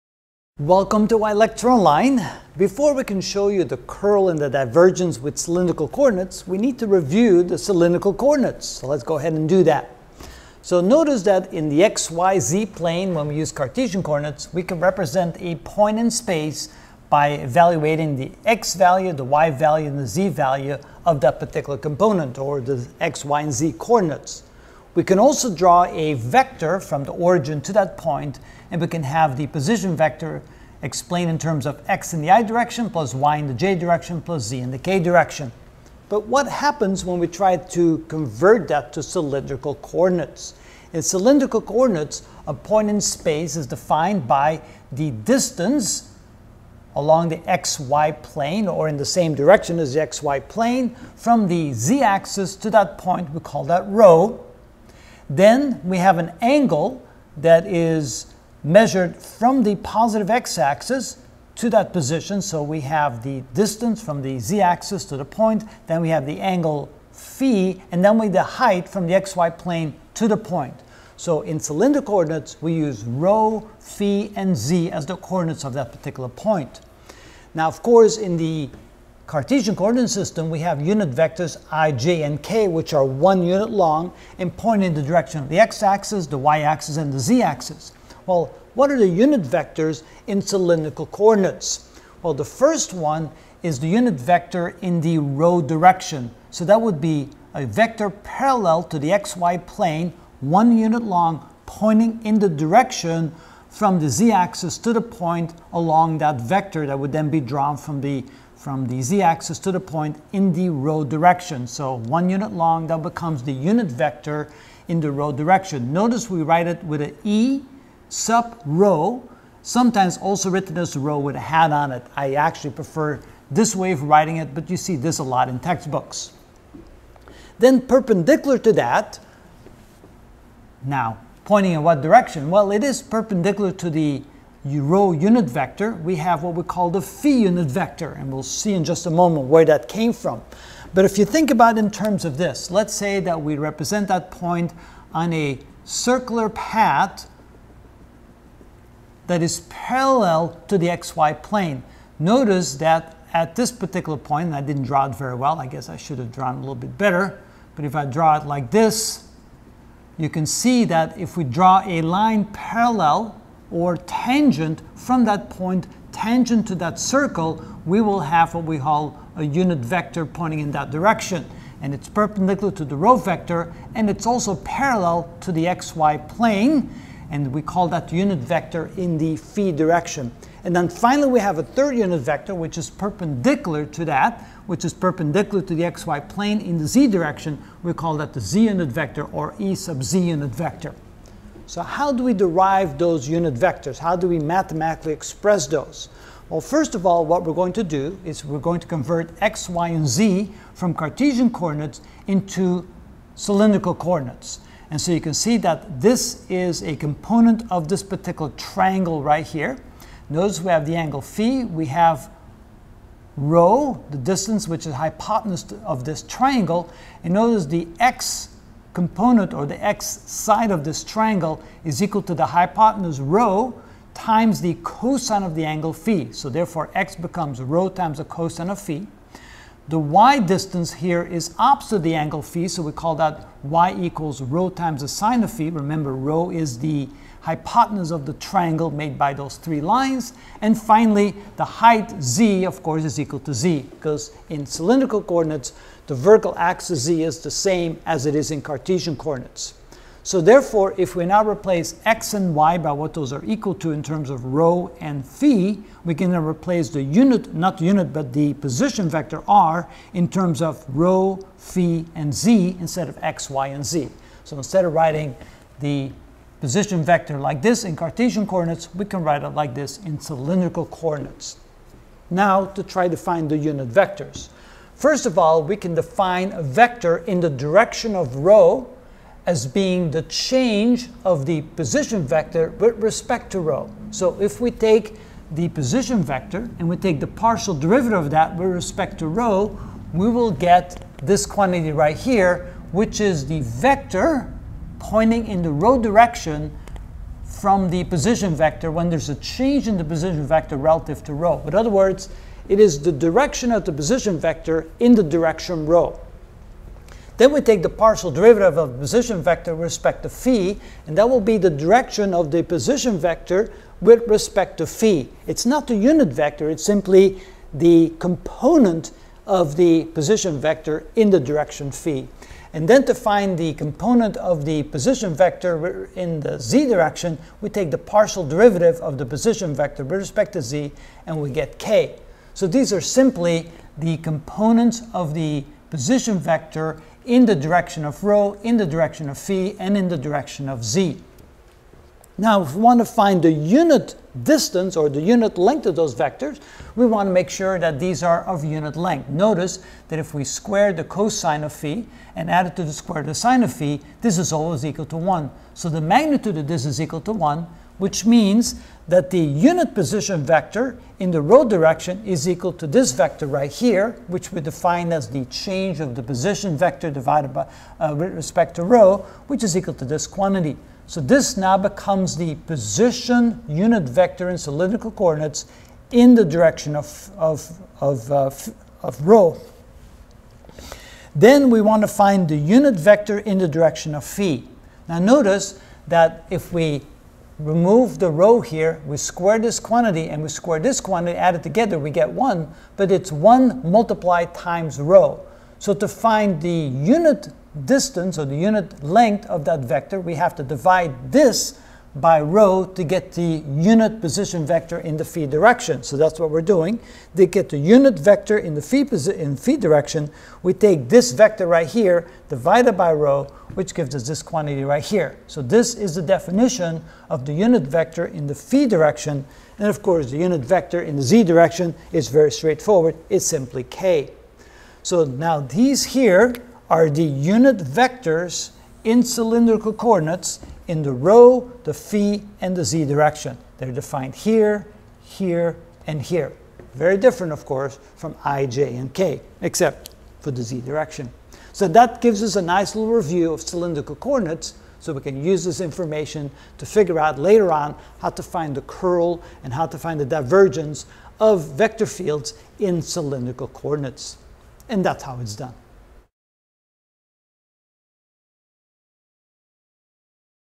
Calculus 3: Divergence and Curl (33 of 50) Cylindrical Coordinates
In this video I will review the cylindrical coordinates and its relationship to Cartesian coordinates.